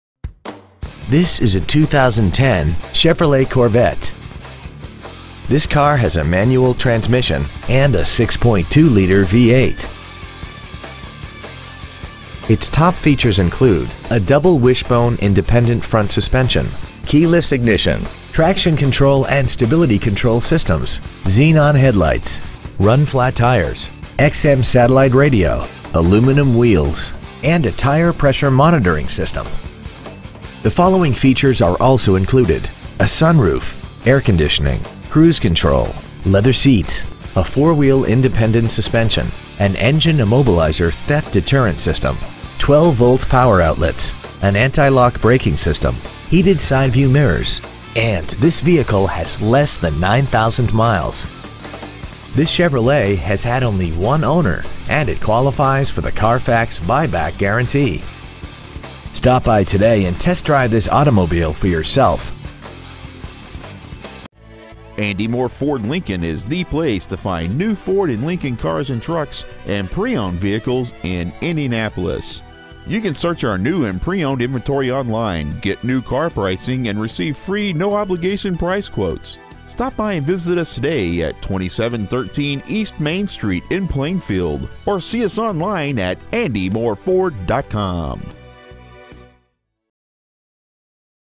2010 Chevrolet Corvette Plainfield IN 46168
Year : 2010
 Make : Chevrolet
 Model : Corvette
 Engine : 6.2L V8 16V MPFI OHV
 Trans . : 6-Speed Manual
 Exterior : Blade Silver Metallic
 Miles : 8,114
 Interior : Ebony
 Stock : C12787A

 2713 E. Main Street